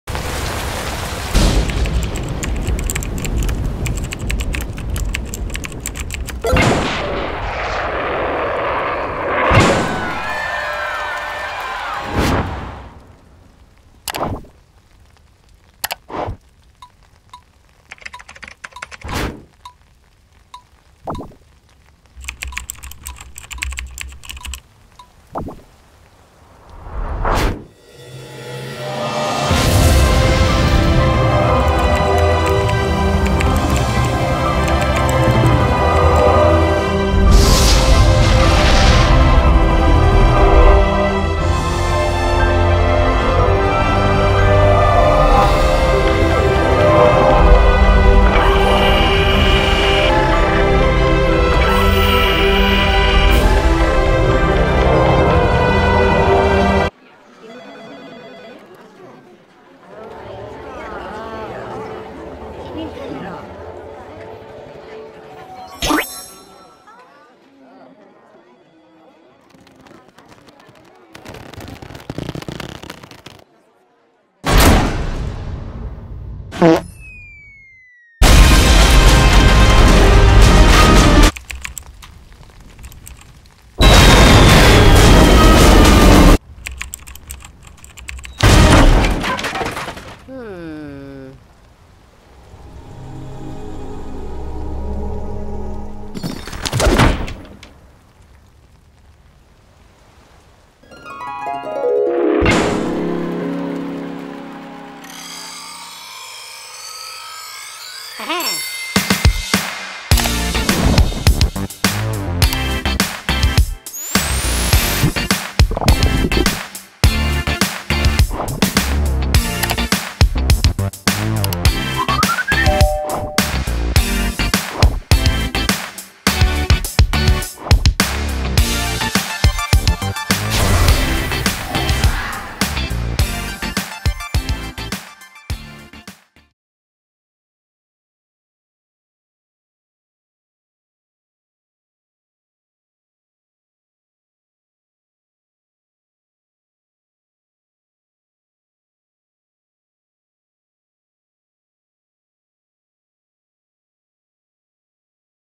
Upload Custom Stickers in Boosted Servers
Show off everything that makes your community special with custom stickers, now available on all Boost Level 1 servers.